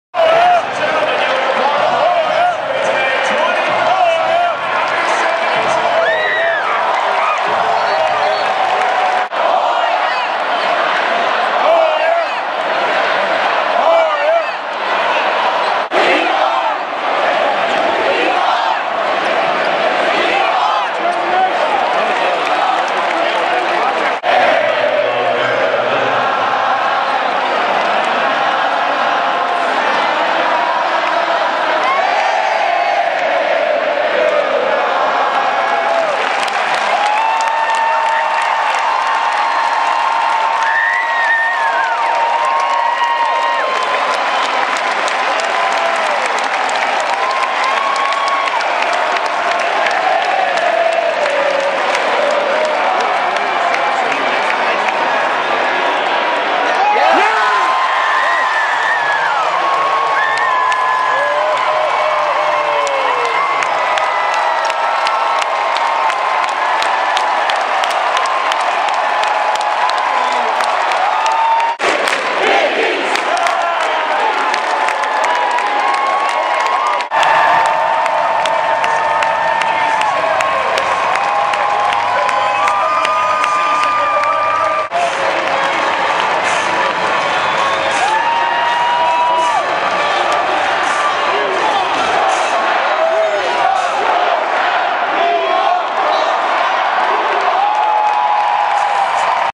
Georgetown/Syracuse March 9 - Fan Highlights
A few highlights from sitting in the lower bowl during the Syracuse at Georgetown men's basketball game on March 9.